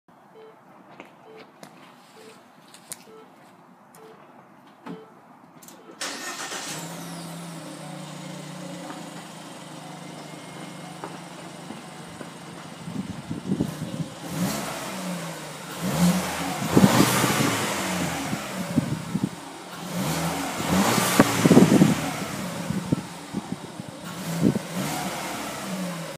2013 Ford Escape 1.6L EcoBoost Exhaust Clip
New Escape alright engine a bit of a dog for a 1.6L, Nicer in the Fusion.